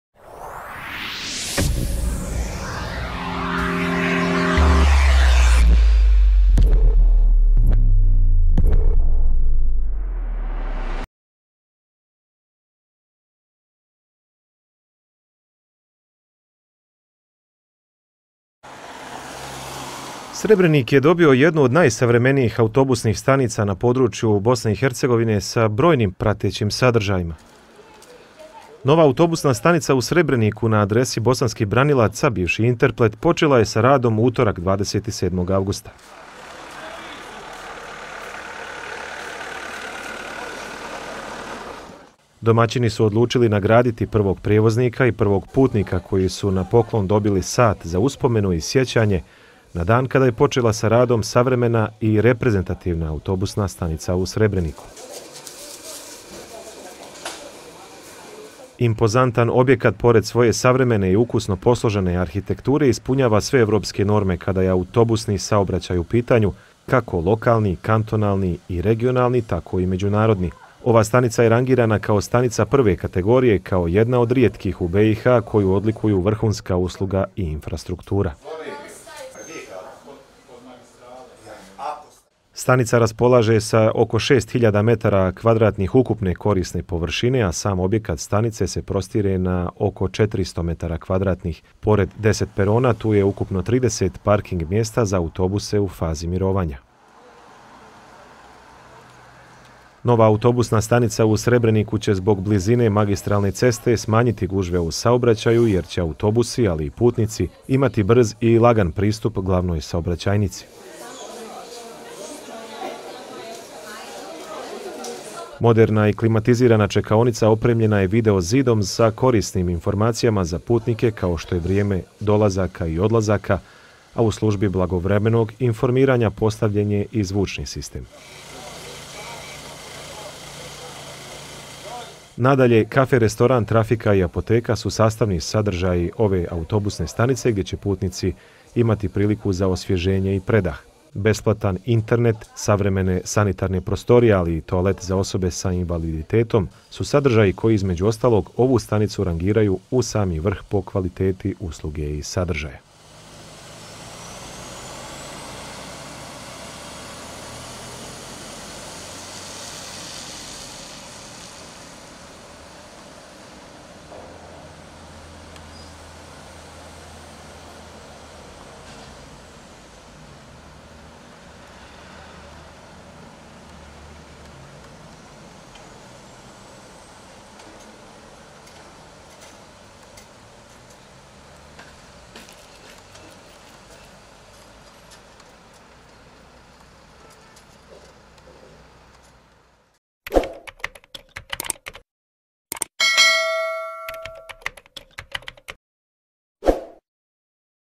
Nova autobuska stanica Srebrenik počela sa radom - tatabrada.tv
Nova autobusna stanica u Srebreniku na adresi Bosanskih branilaca (bivši “Interplet”) počela je sa radom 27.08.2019. Ova stanica rangirana je kao stanica 1. kategorije, kao jedna od rijetkih u BiH koju odlikuju vrhunska usluga i infrastruktura.